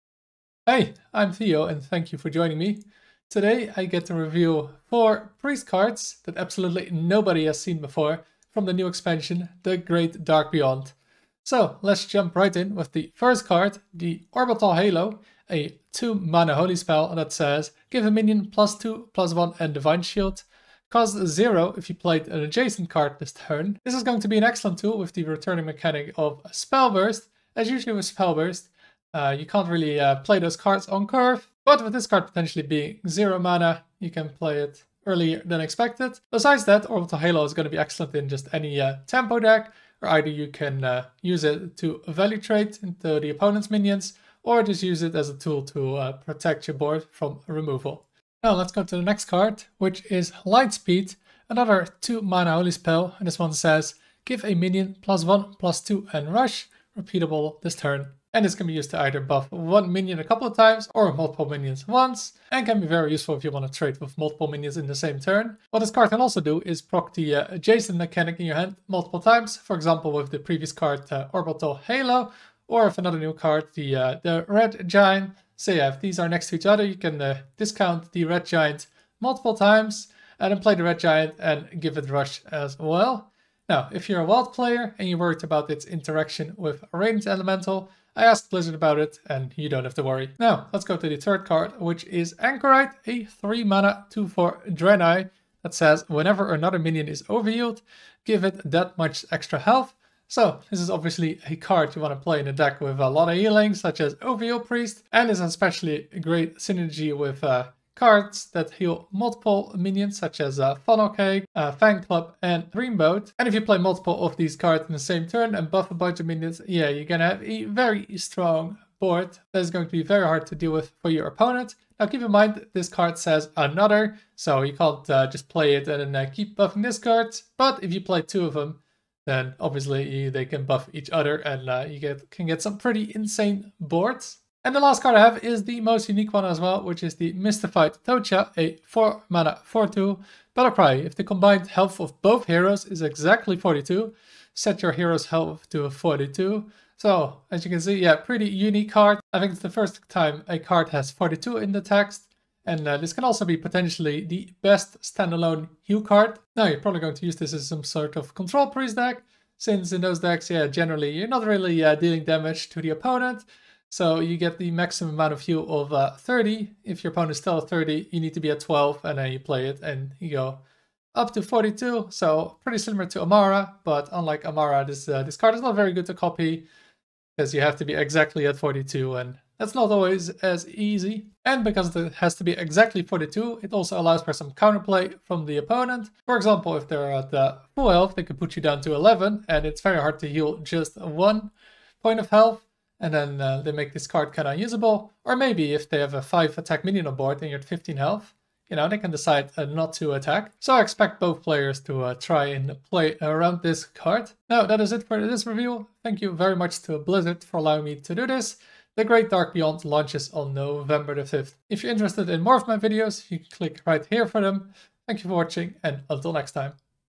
4 Mana Heal 30?! Revealing 4 Priest Cards From The Great Dark Beyond!
Today we reveal 4 new Priest cards for Hearthstone's next expansion The Great Dark Beyond, which launches November 5th!